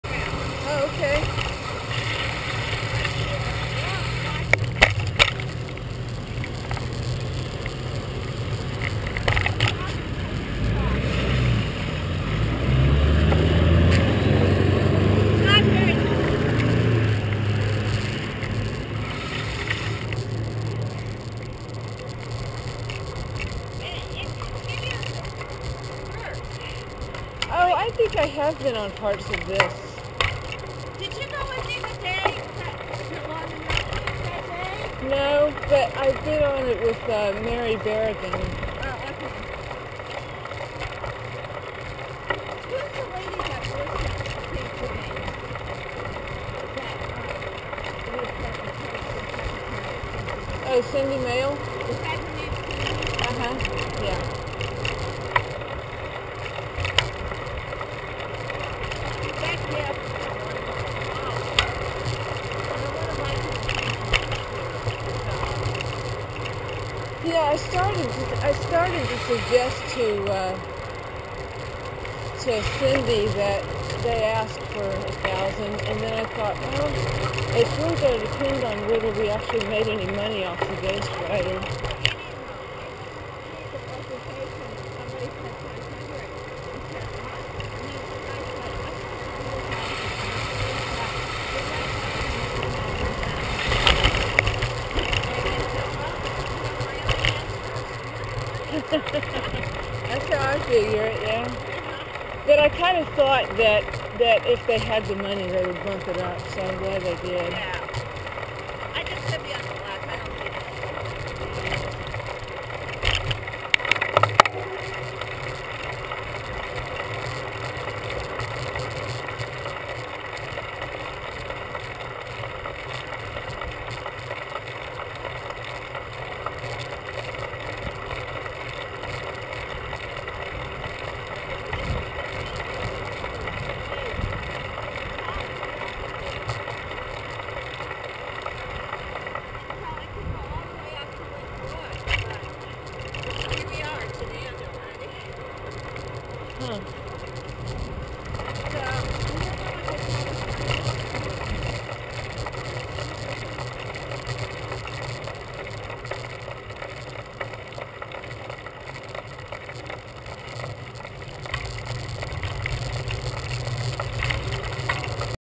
Riding the new Mill Creek Trail
Sampling the new Mill Creek Trail portion that parallels Caldwell Nature Preserve. It would be great if we had a trail that ran the entire length of the Mill Creek from north to south.